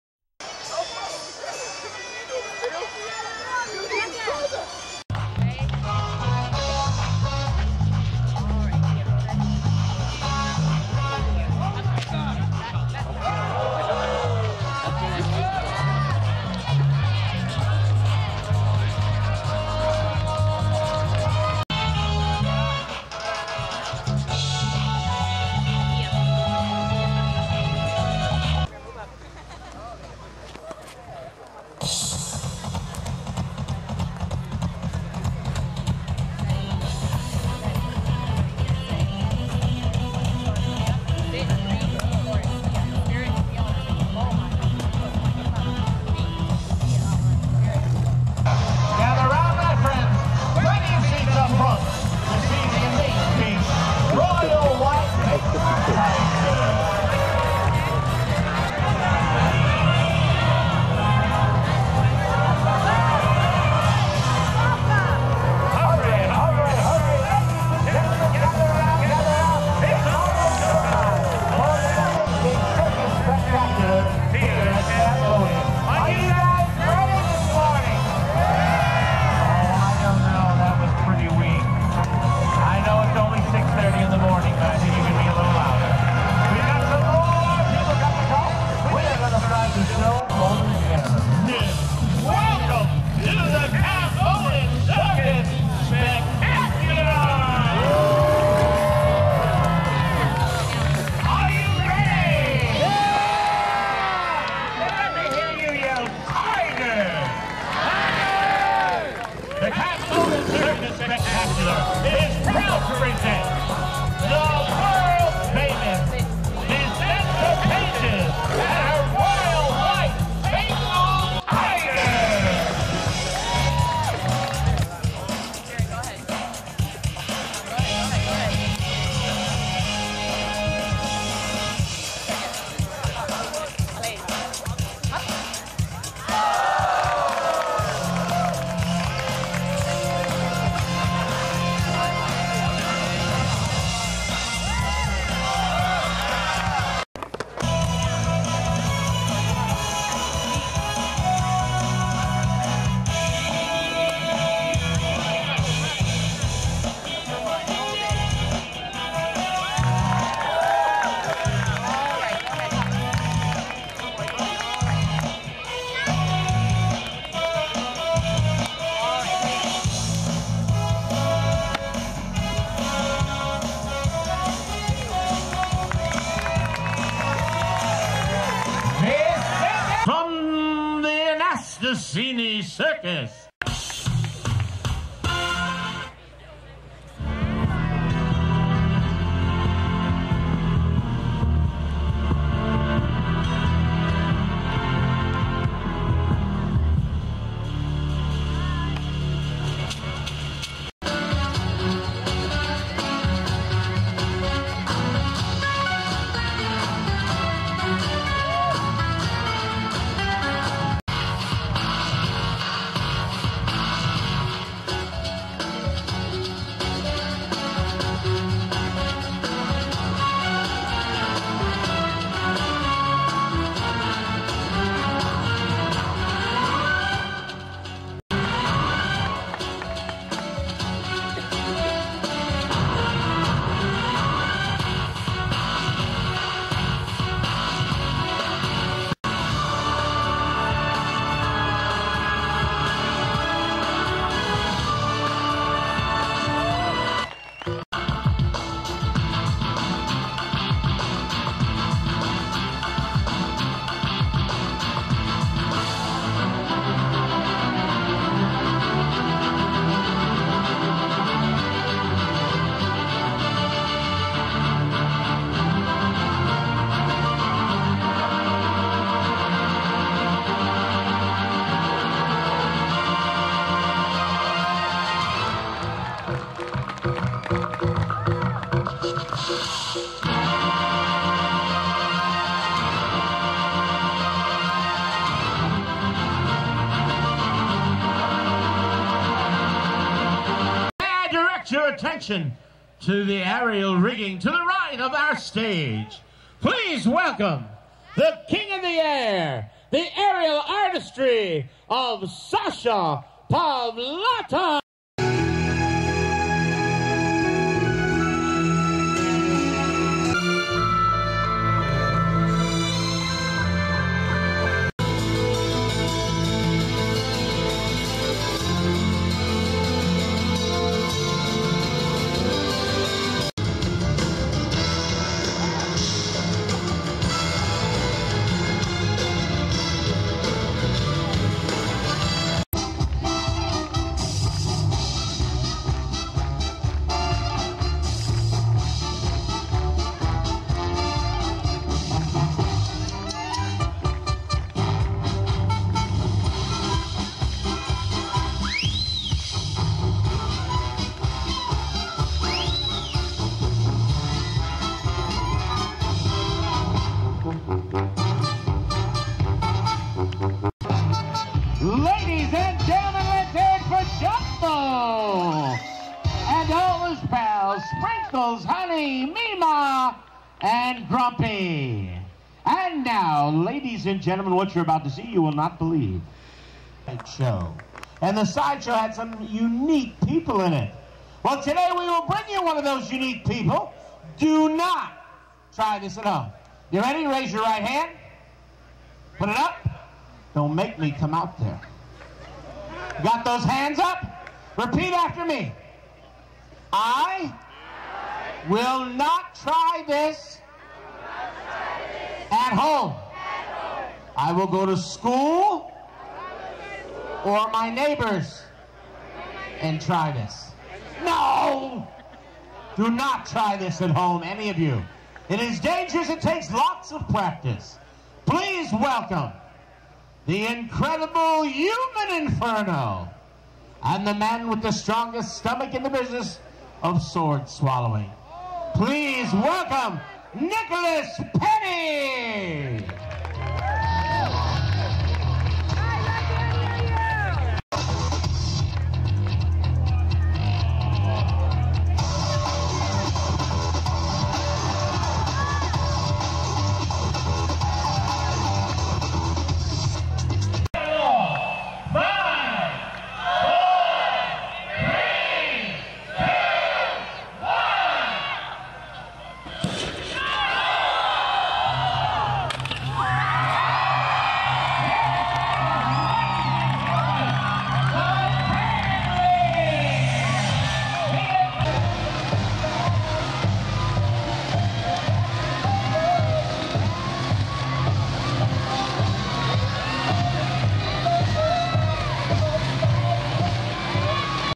The Amazing Anastasini - Secret Circus
We were called by a Top Secret Organization to bring together some of the World's Greatest Acts. But the catch was, we were contracted not to tell anyone of the performance or the secret location. so secret that it makes Area 51 more popular than Disney World!

Performers needed to sneak in under the cover of Darkness, with others starting to arrive @ 2AM for the 6AM Secret Show. yes that is AM this is not a typo!

Giovanni Anastasini Produced a beautiful Show only to be witnessed by a Chosen Few. Vicenta Pages & Her White Bengal Tigers; the 9th Generation Acrobatic Artistry of Chiara Anastasini; "The King of the Air" - Sacha Pavlata; Clowns, Clowns & more Clowns than any touring Show in the united States; "The Human Inferno" - Nicholas Penney; The Cannon Lady - Jennifer Smith; and Larry Records and his Herd of Camels.

Such a Great Show, it was a shame it had to be a secret in an undisclosed location. Yes and a secret it must be, but if you would like any of these Fantastic Features you can have them at your next event; one or all; Secret or not!

Call Gordon or Marybeth MacKay @ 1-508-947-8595 for information on booking any of the acts seen in this video.